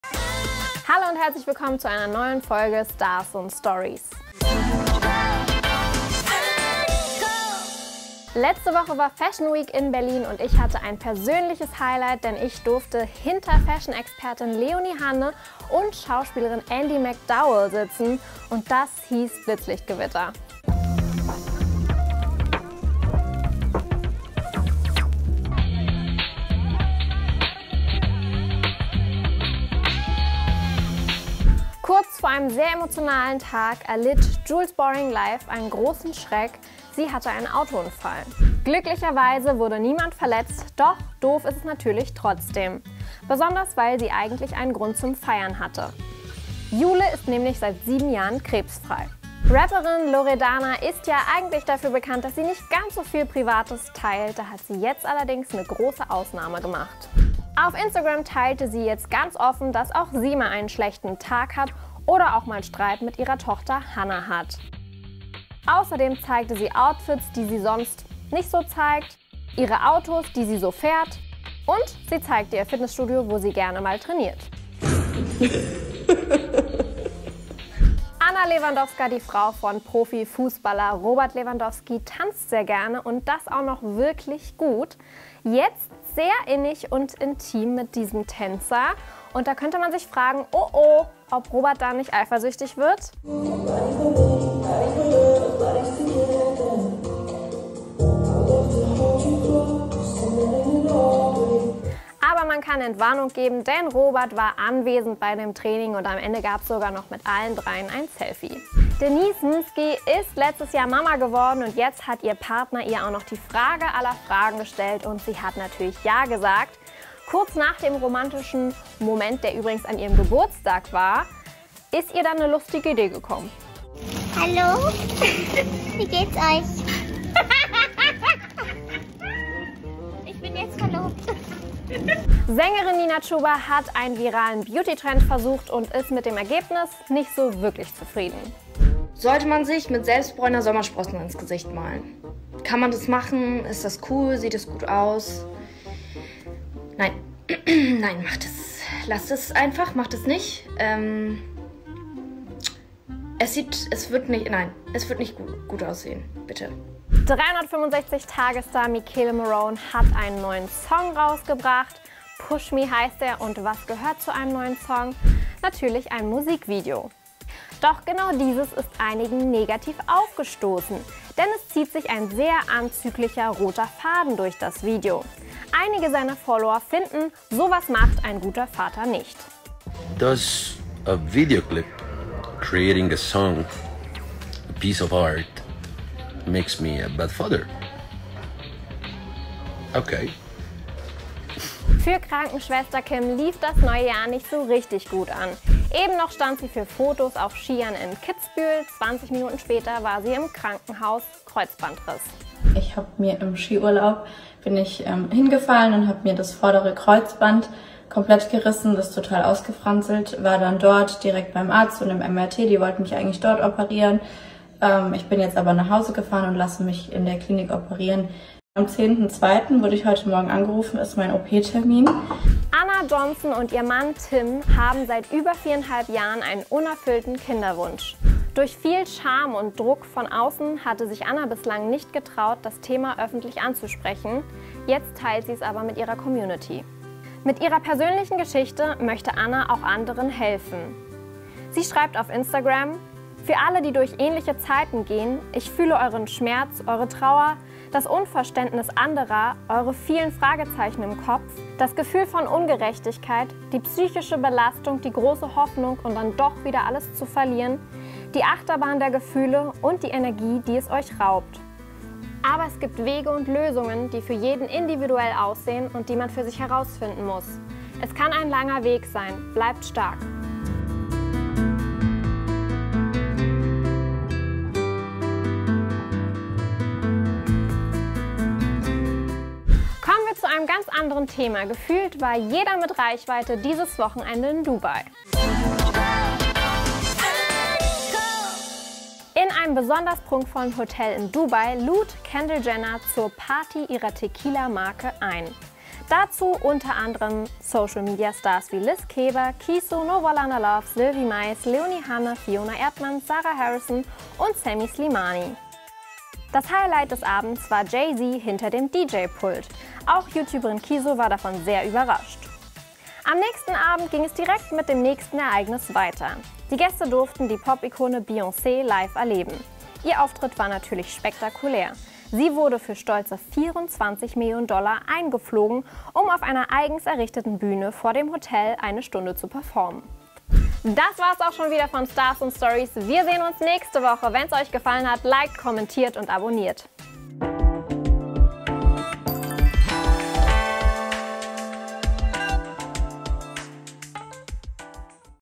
Ana & Tim teilen private Einblicke | STARS & STORIES #50
Seit über 4,5 Jahren haben Ana & Tim Johnson einen unerfüllten Kinderwunsch, Michele Morrone hat Stress mit seinen Fans und wir fassen nochmal zusammen, wieso alle gerade in Dubai waren!
In diesem Video:
0:00 - Intro
0:08 - Fashionweek Berlin
0:32 - JulesBoringlife
0:50 - Loredana
1:18 - Anna Lewandowska
1:49 - Denise Mski
2:13 - Nina Chuba
2:38 - Michele Morrone
3:13 - Kim Schiele
3:43 - Ana Johnson
4:40 - Dubai: Kendall Jenner & Beyoncé